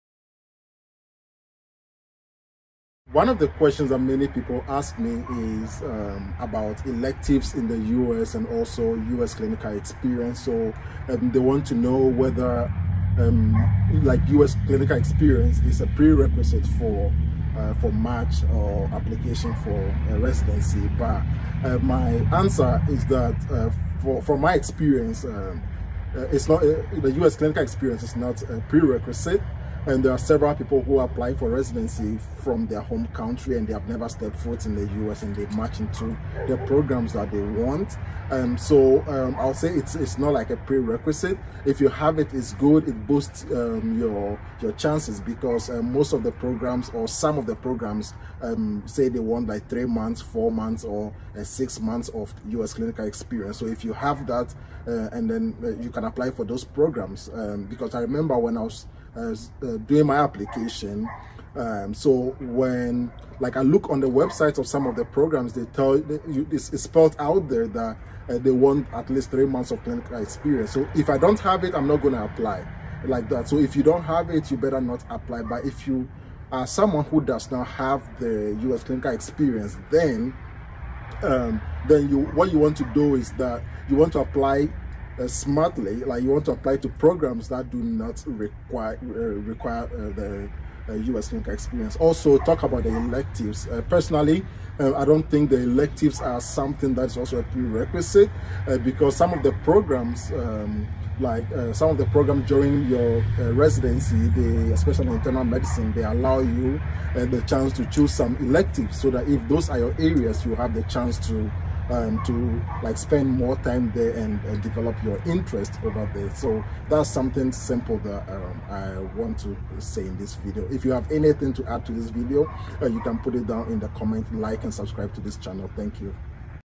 Is Doing Electives in America and US Clinical Experience Necessary for Residency? USMLE BOOSTER
Electives in America, US Clinical Experience Necessary for Residency? USMLE BOOSTER